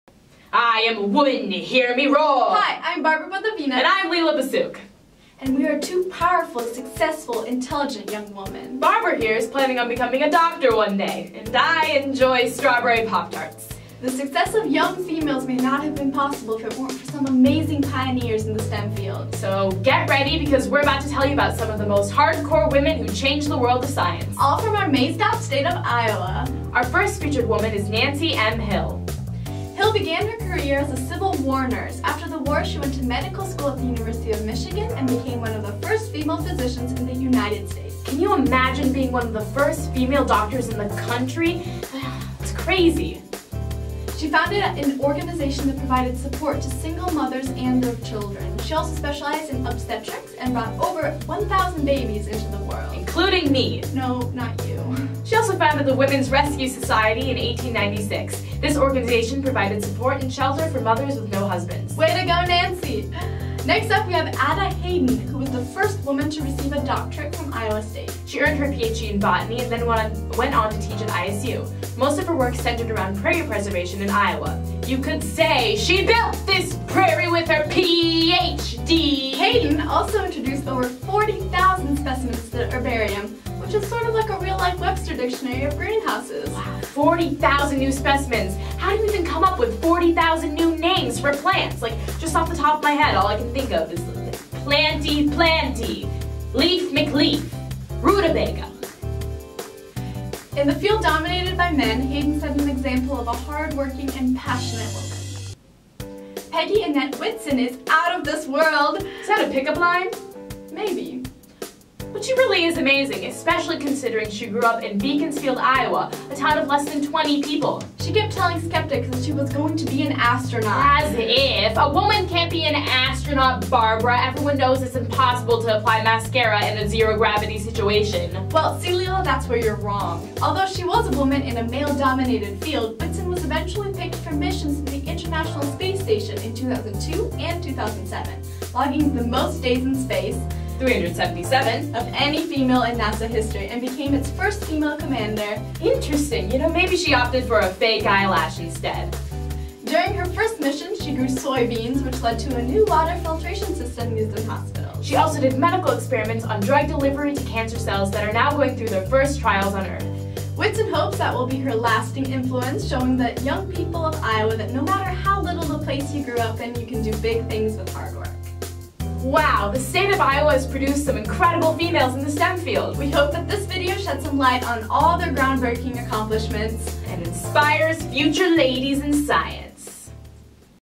Women of STEM from Iowa
Amazing Women in STEM from the maized out state of Iowa
thanks to Carolyn Walling, Barbara Badovinac, Leela Bassuk and Layla Hannaford